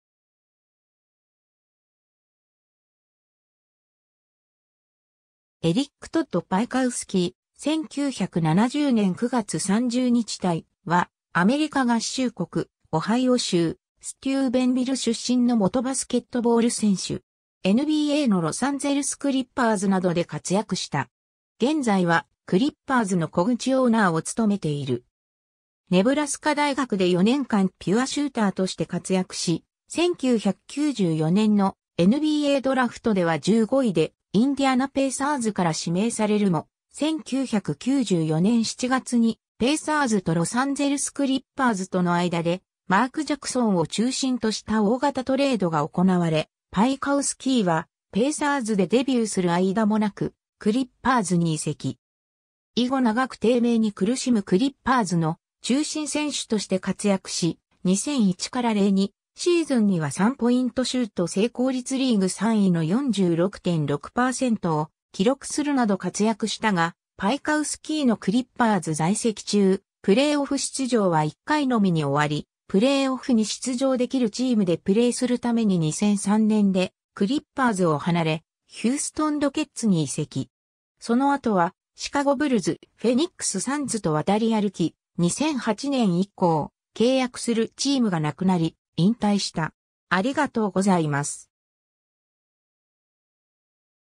エリック・パイカウスキー
エリック・トッド・パイカウスキー（Eric Todd Piatkowski（発音：[ˌpaɪ.ətˈkaʊski]）、1970年9月30日 - ）は、アメリカ合衆国・オハイオ州ステューベンビル出身の元バスケットボール選手。
 NBAのロサンゼルス・クリッパーズなどで活躍した。
現在はクリッパーズの小口オーナーを務めている。
 ネブラスカ大学で4年間ピュアシューターとして活躍し、1994年のNBAドラフトでは15位でインディアナ・ペイサーズから指名されるも、1994年7月にペイサーズとロサンゼルス・クリッパーズとの間でマーク・ジャクソンを中心とした大型トレードが行われ、パイカウスキーはペイサーズでデビューする間もなくクリッパーズに移籍。
以後長く低迷に苦しむクリッパーズの中心選手として活躍し、2001-02シーズンには3ポイントシュート成功率リーグ3位の46.6%を記録するなど活躍したが、パイカウスキーのクリッパーズ在籍中、プレーオフ出場は1回 (1996年) のみに終わり、プレーオフに出場出来るチームでプレーするために2003年でクリッパーズを離れ、ヒューストン・ロケッツに移籍。
その後はシカゴ・ブルズ、フェニックス・サンズと渡り歩き、2008年以降契約するチームが無くなり、引退した。